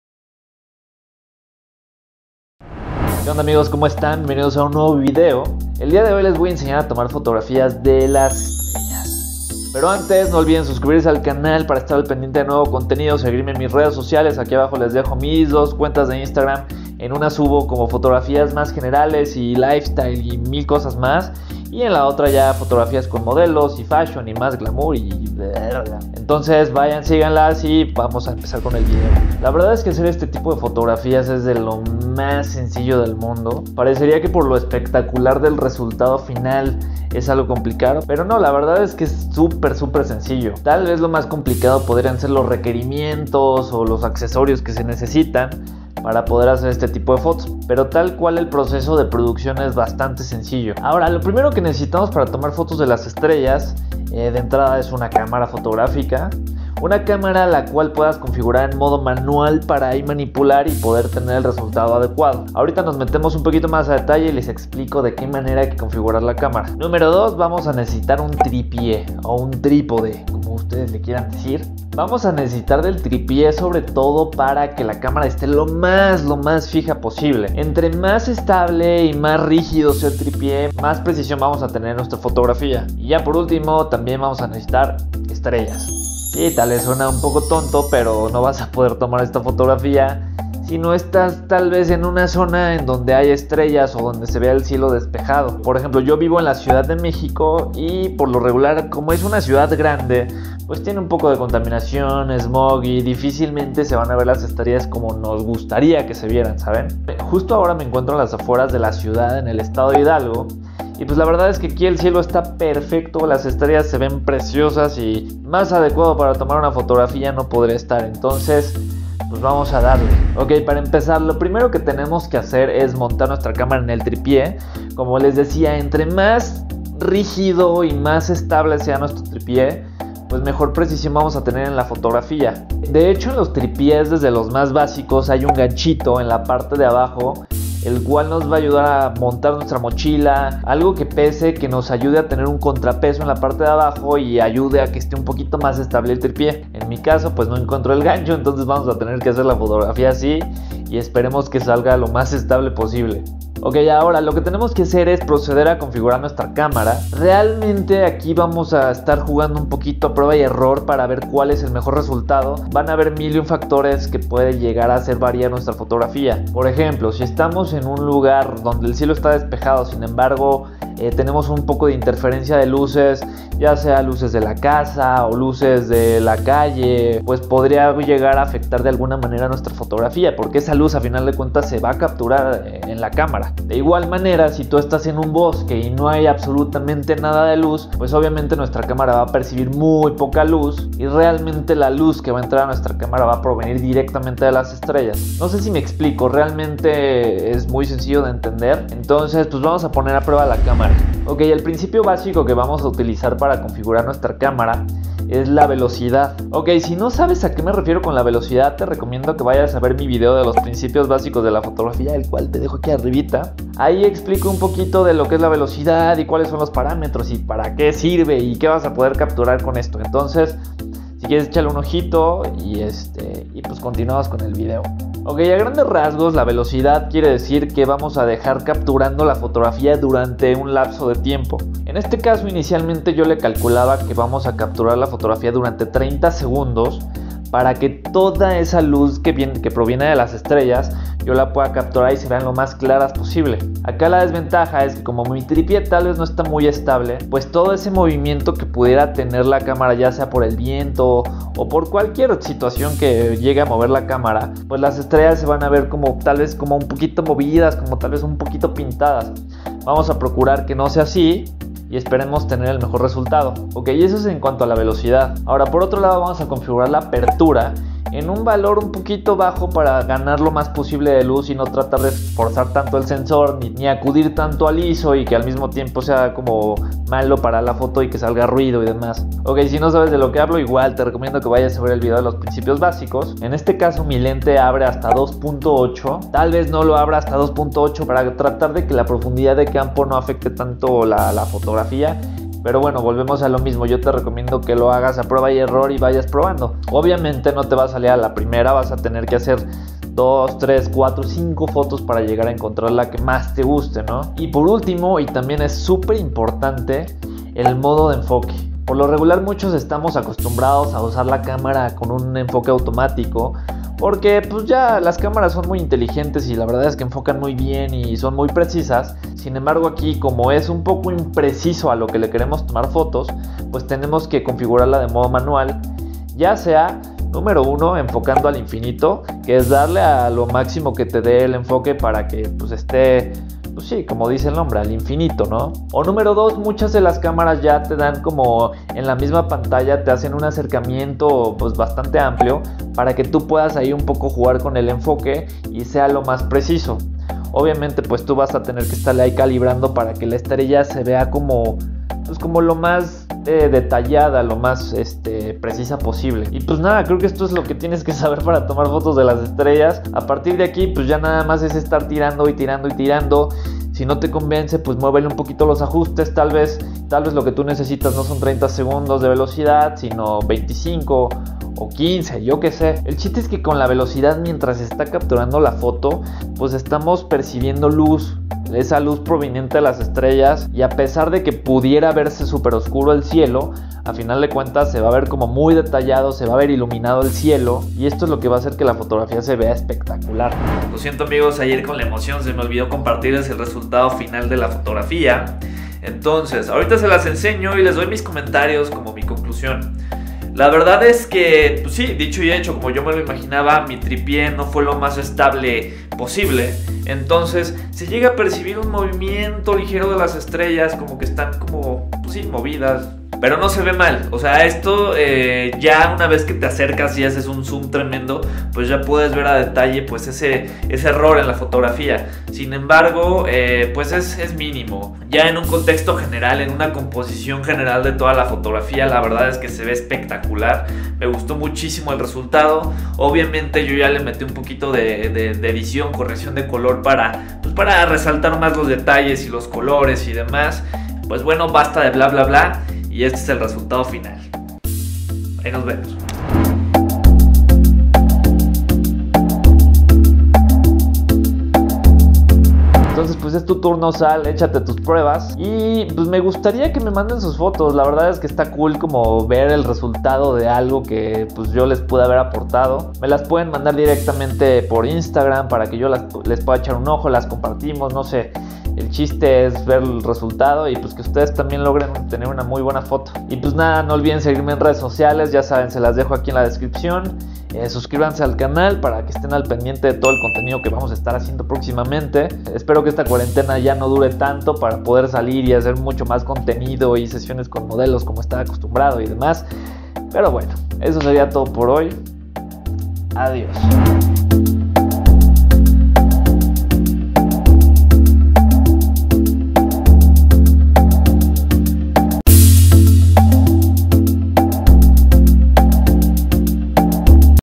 Cómo tomar fotos a las estrellas ⭐🌟🌠
En este video les enseño como tomar fotografías a las estrellas y el espacio.
Solo necesitamos una cámara fotográfica que se pueda configurar en modo manual y un tripie.